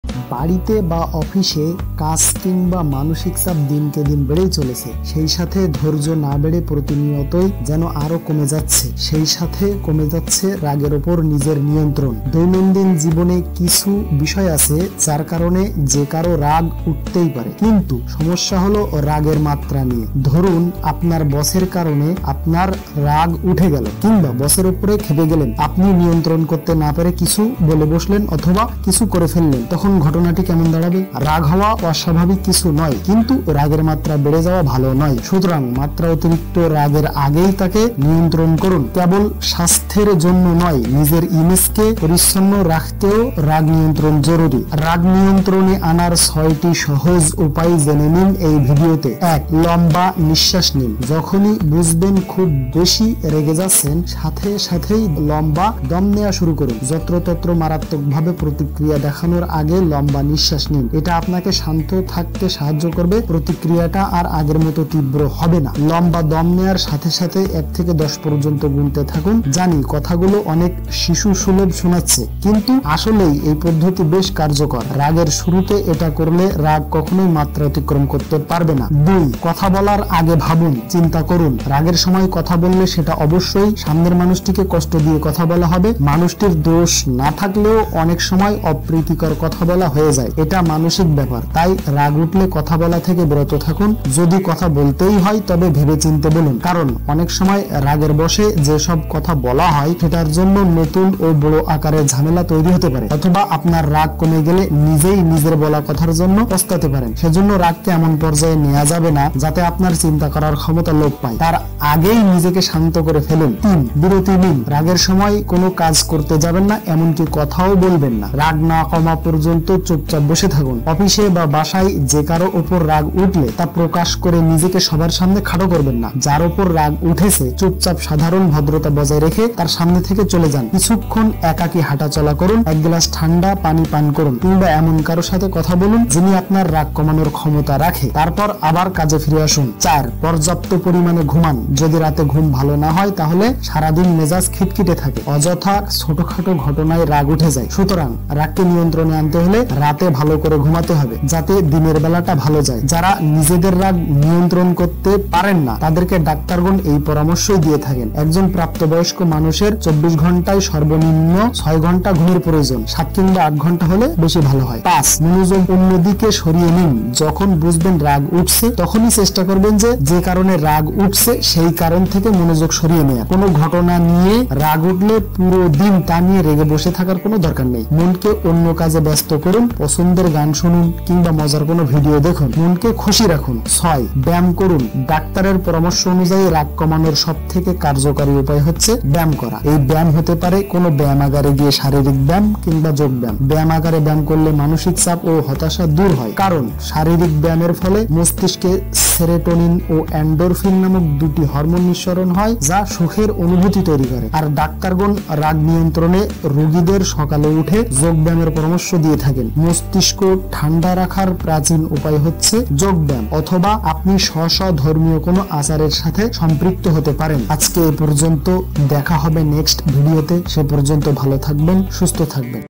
রাগ নিয়ন্ত্রণ করার ৬টি অব্যর্থ কৌশল!
বাড়িতে বা অফিসে কাজ কিংবা মানসিক চাপ দিনকে দিন বেড়েই চলেছে। সেই সাথে ধৈর্য না বেড়ে প্রতিনিয়ত যেন আরো কমে যাচ্ছে। সেই সাথে কমে যাচ্ছে রাগের ওপর নিজের নিয়ন্ত্রণ। দৈনন্দিন জীবনে কিছু বিষয় আছে, যার কারনে যে কারো রাগ উঠতেই পারে। কিন্তু সমস্যা হলো রাগের মাত্রা নিয়ে।